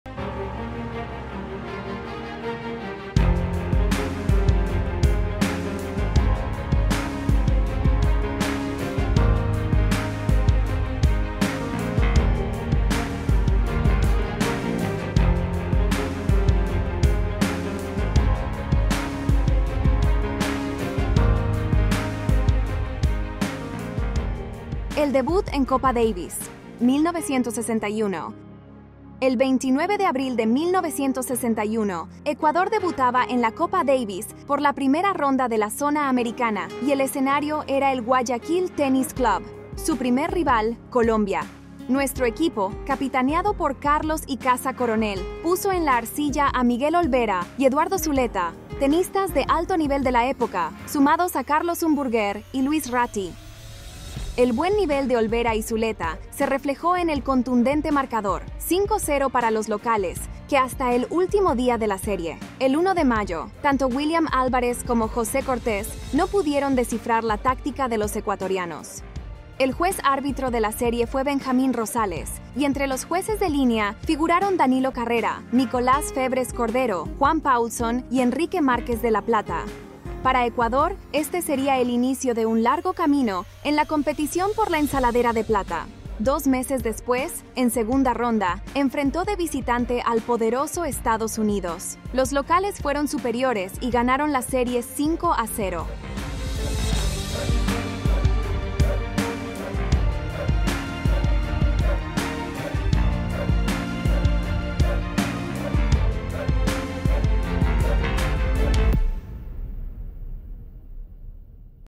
BAÚL TENIS… Recordando nuestra historia: El debut en Copa Davis, 1961
El 29 de abril de 1961, Ecuador debutaba en la Copa Davis por la primera ronda de la Zona Americana y el escenario era el Guayaquil Tenis Club. Su primer rival, Colombia. Nuestro equipo, capitaneado por Carlos Ycaza Coronel, puso en la arcilla a Miguel Olvera y Eduardo Zuleta, tenistas de alto nivel de la época, sumados a Carlos Humburguer y Luis Ratti…